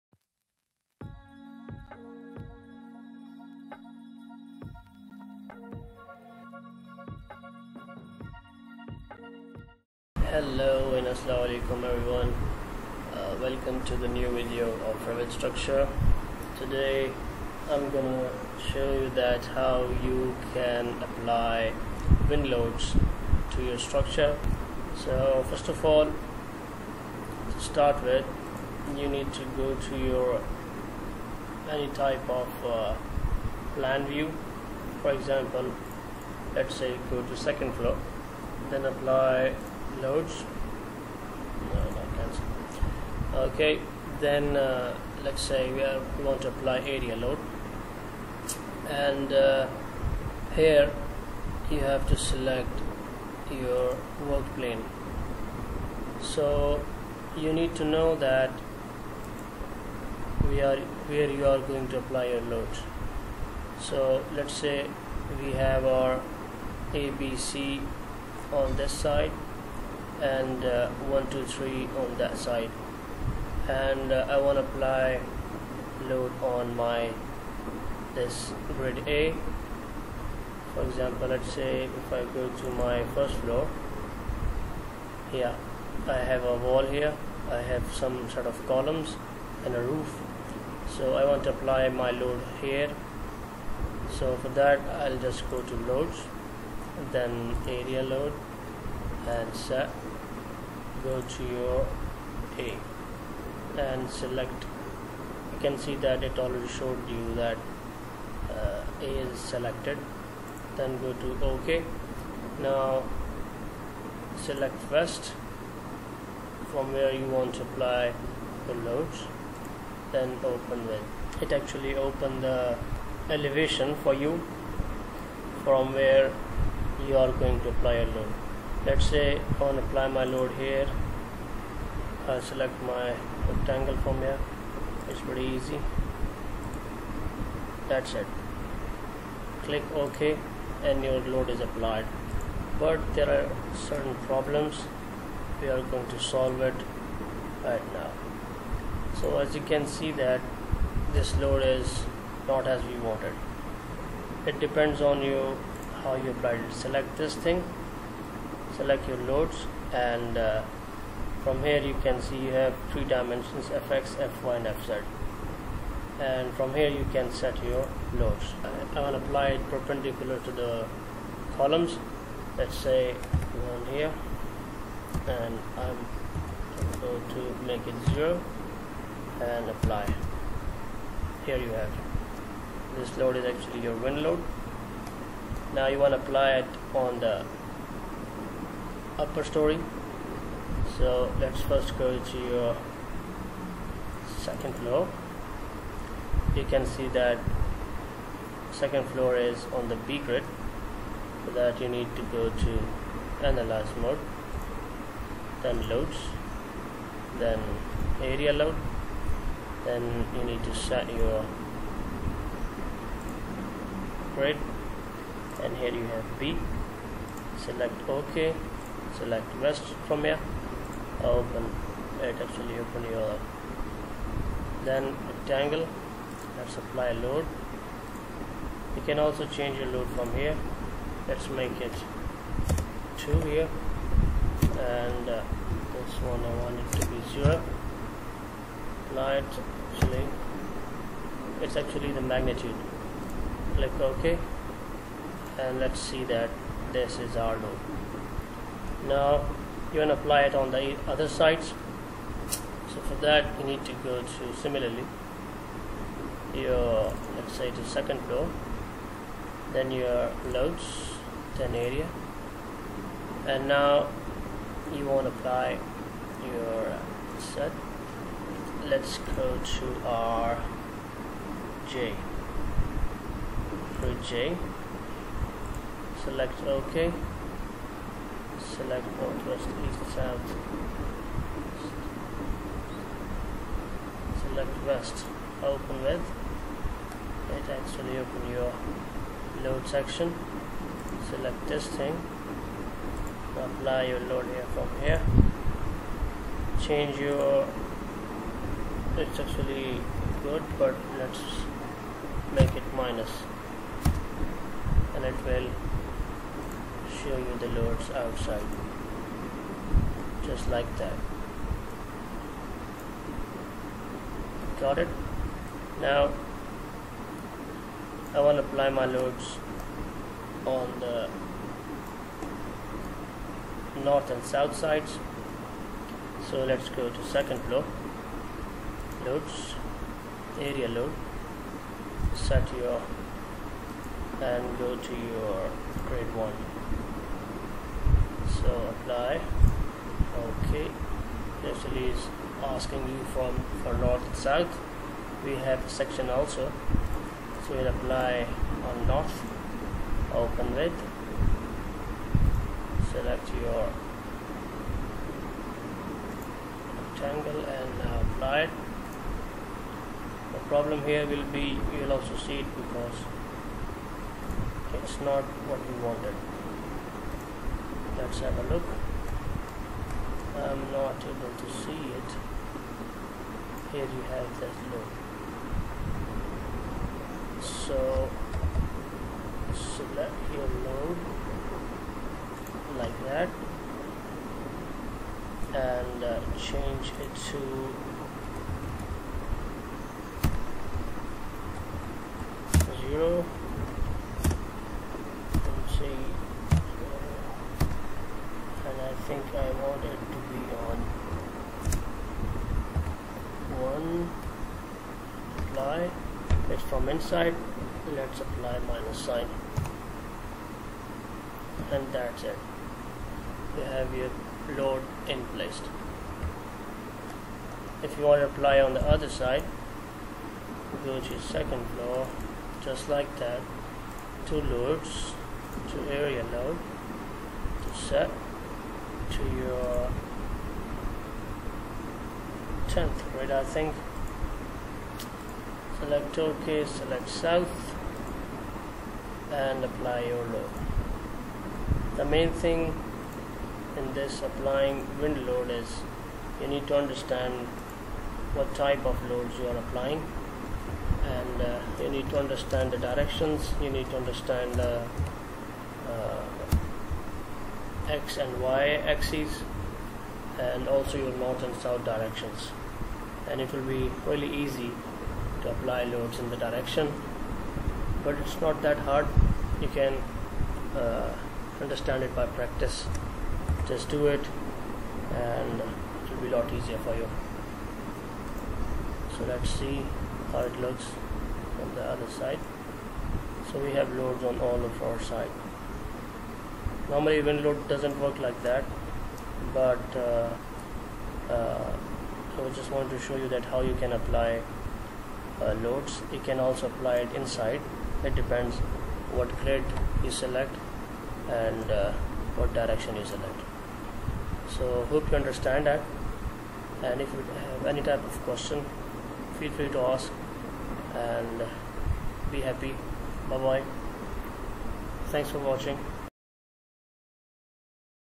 How to apply wind loads in revit
Wind loads are importent in bigger structure but it is importent know how to apply them.